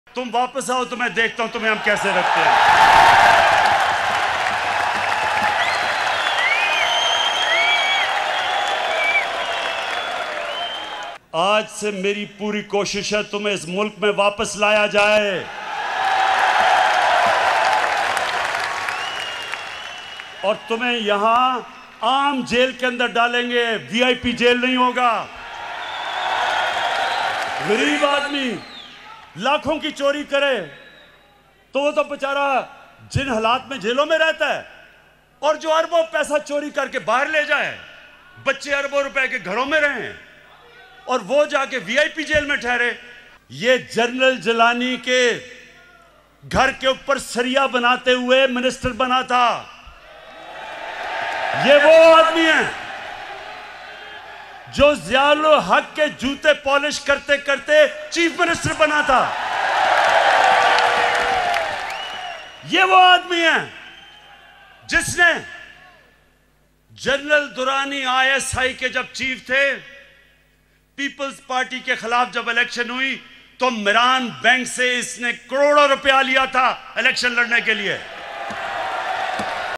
تم واپس آؤ تمہیں دیکھتا ہوں، وزیراعظم
وزیراعظم کا نواز شریف کو لانے کا اعلان